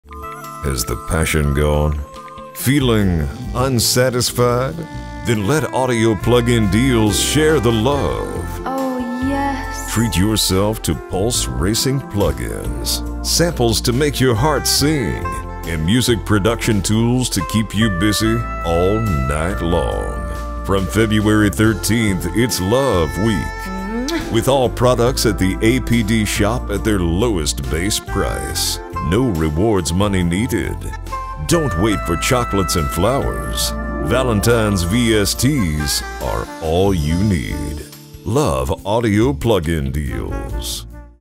Audio Plugin Deals Love Week 2024
Join us at Audio Plugin Deals for this year's Love Week, with all products in the shop offered at base price for the next 1 week!

Love Week starts today, Feb 13th and ends on Feb 20th, 2024 - don't be late to the party!